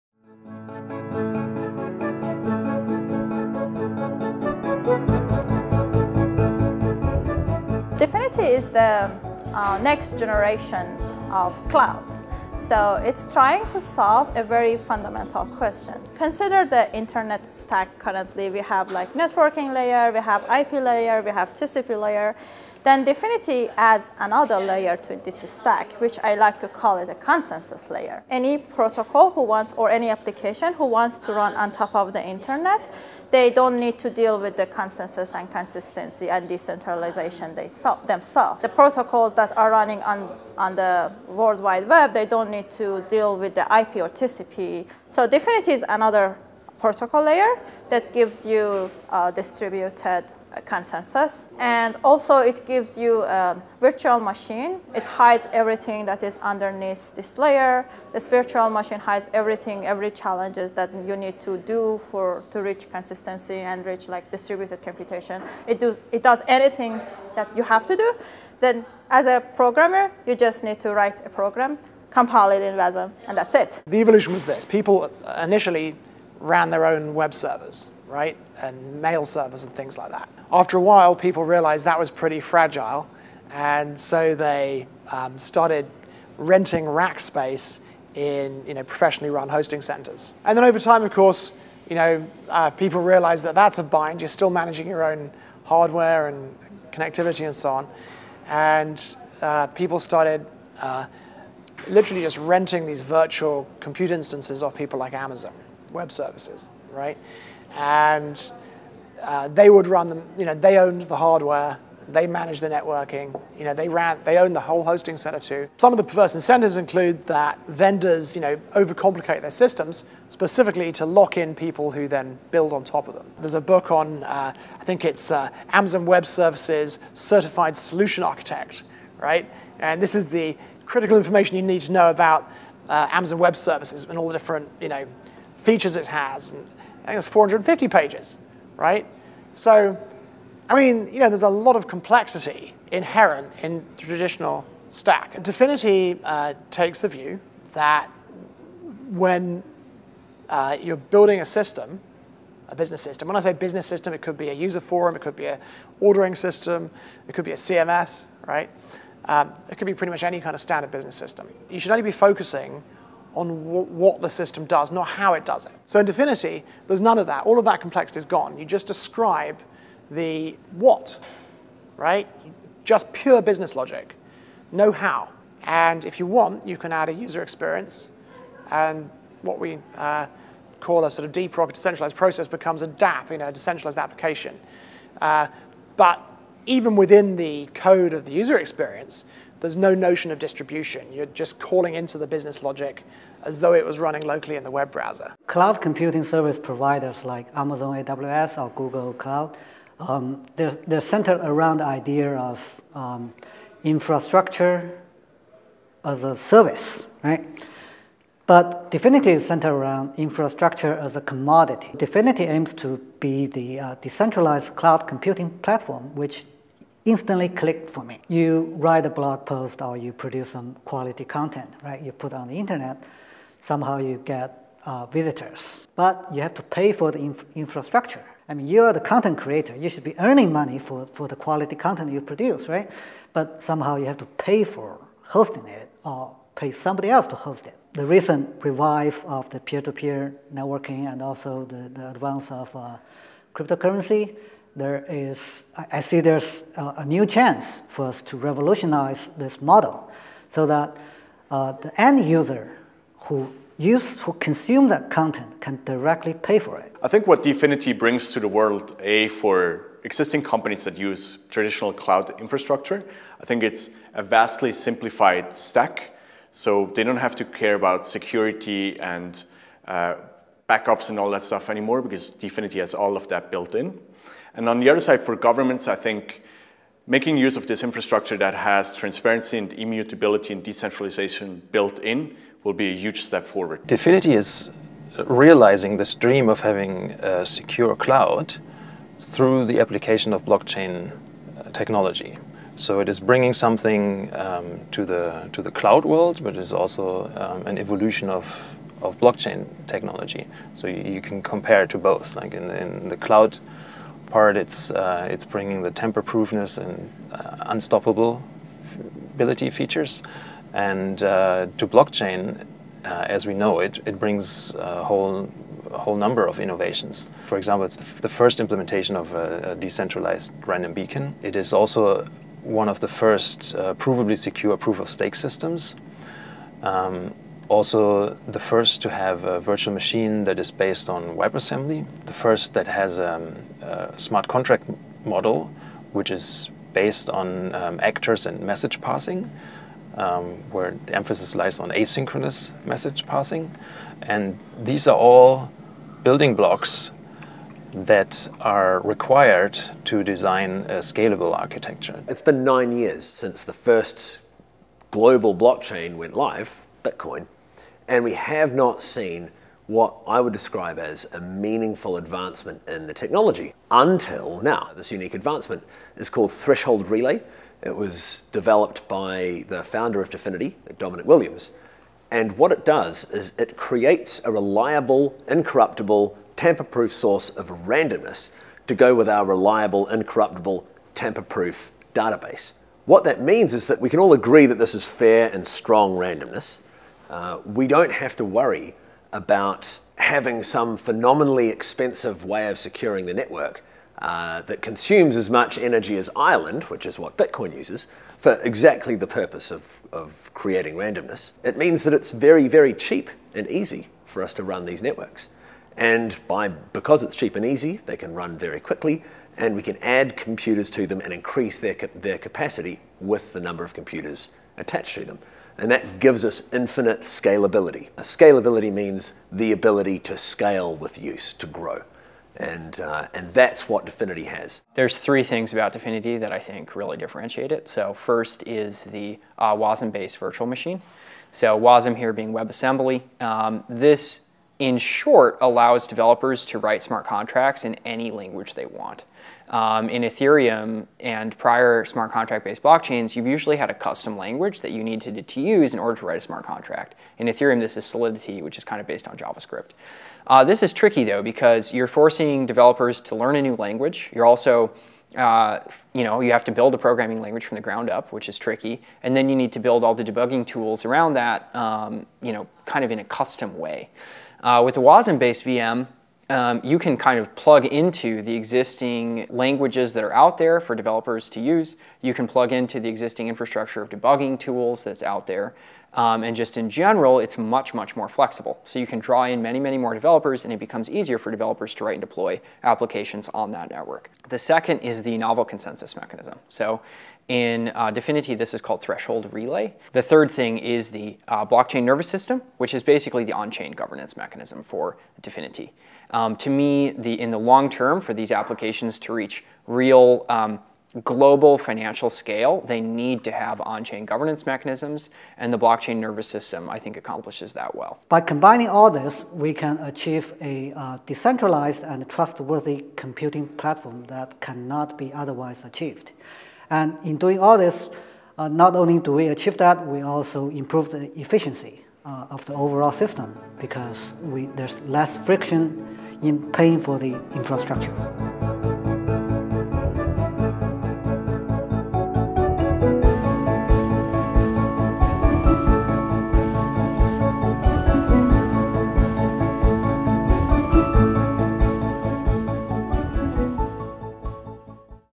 Thoughts on DFINITY
DFINITY is the next generation of cloud: By hiding a consensus layer behind a virtual machine, DFINITY enables application developers to easily access secure distributed computation.

DFINITY takes the view that you should only be focused on what a software system does, not how it does it. This is achieved by discarding the complexity of the tradition model - the database, the hosting, everything. All you do is write the code and the data that was previously stored in the database is now stored as variables in the software.

Technologies employed by DFINITY include: Blockchain, WebAssembly, an actor-based execution model, Threshold Relay powered random beacon, Probabilistic Slot Consensus, Proof of Stake (PoS) incentive system.